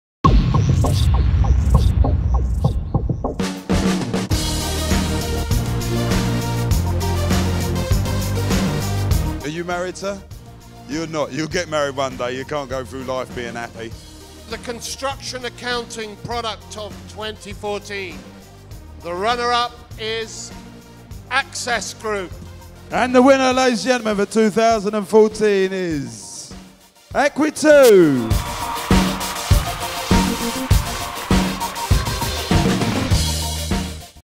Construction Computing Awards 2014: Construction Accounting Product of 2014
Construction Accounting Product of 2014
Winner: Eque2 for EVision
Runner Up: Access Group - Access Dimensions